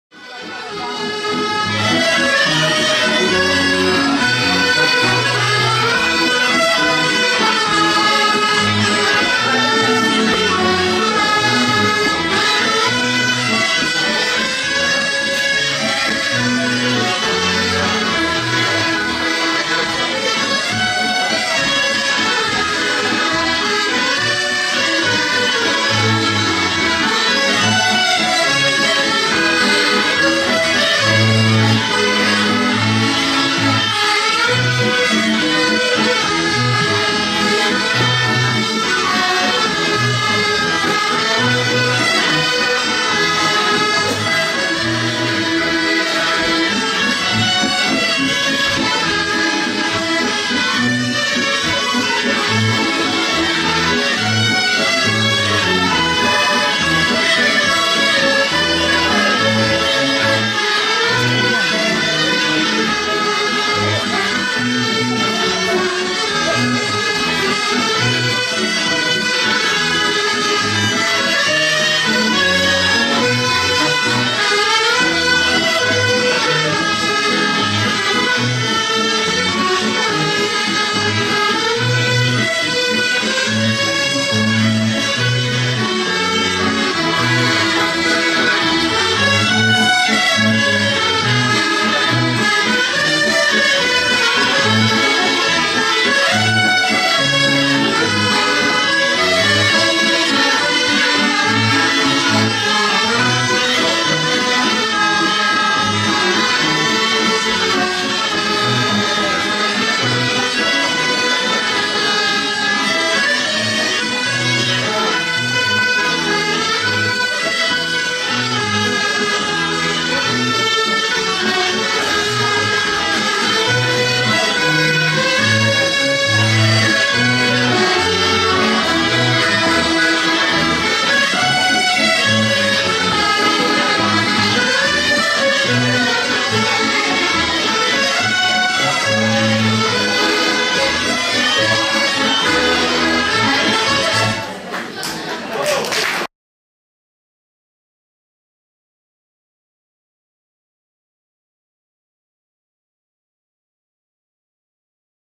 LAVASTRIE mars 2015 Valses et valseurs
LAVASTRIE (15) 29 mars 2015 Banquet des Musiciens de la Haute Auvergne au restaurant MALLET/Vidéo et réalisation Alexandra ASTORG (Sandy)